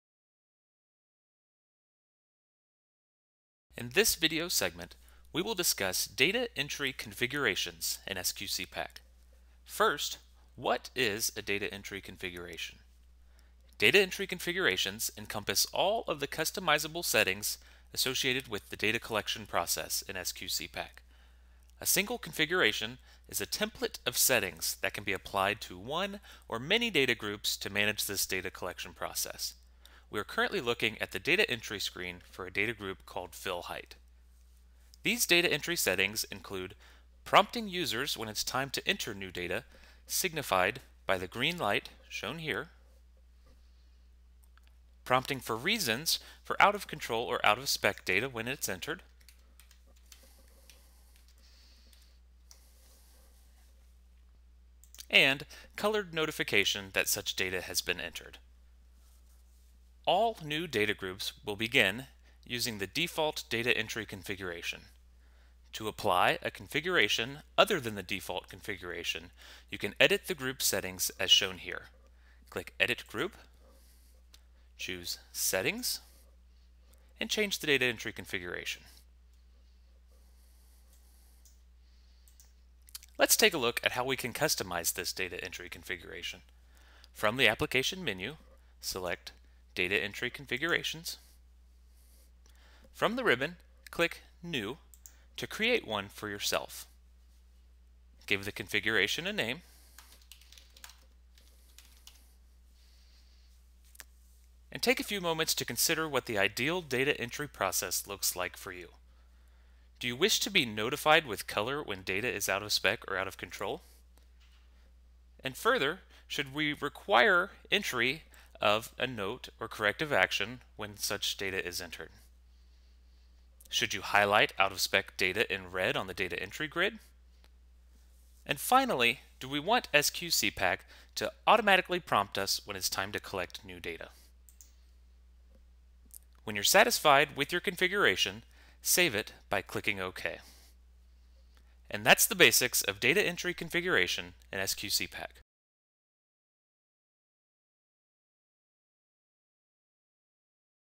SQCpack - Data Entry Configurations
Configuring settings to manage the data collection process.